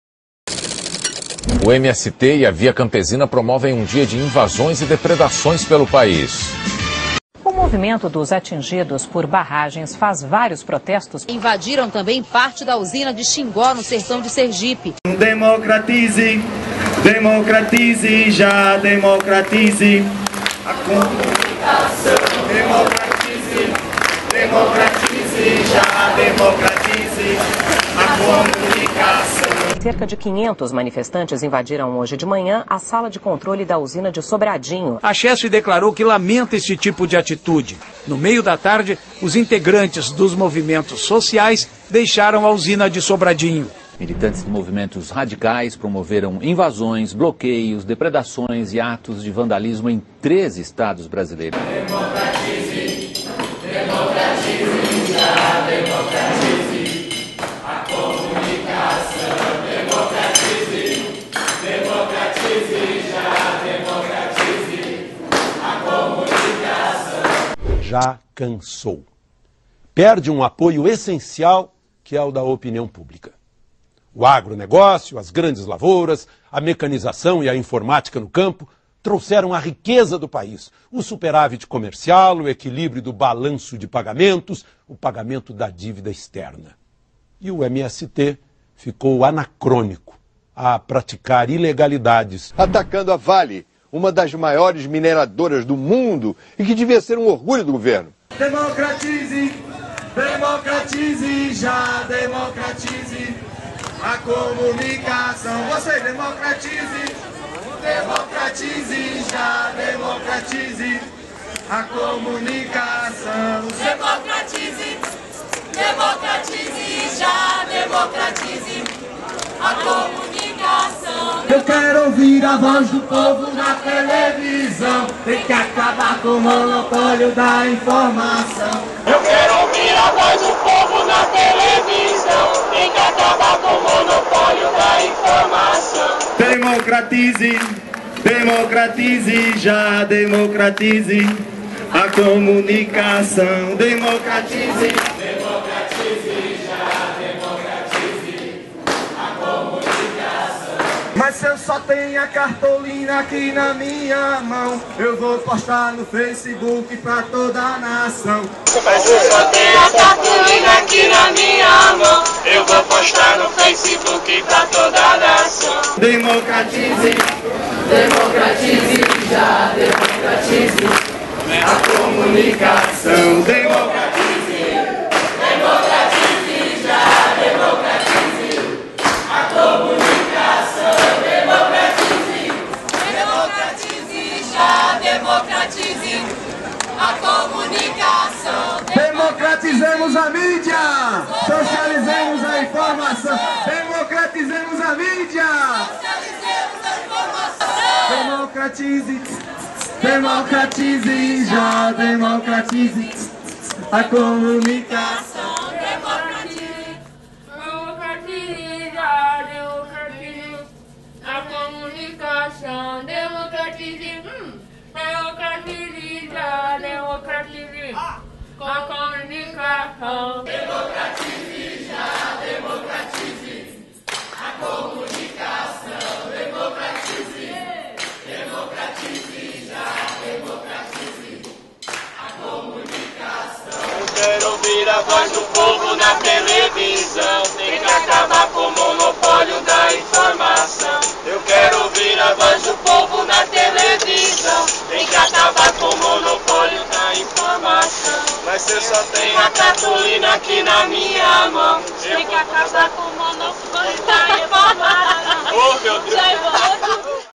DEMOCRATIZE JÁ!
Vídeo realizado pelo setor de comunicação do Movimento dos Atingidos por Barragens (MAB) durante a II Etapa do curso de comunicação no ano de 2013.

DEMOCRATIZEMOS A MÍDIA, DEMOCRATIZEMOS A INFORMAÇÃO!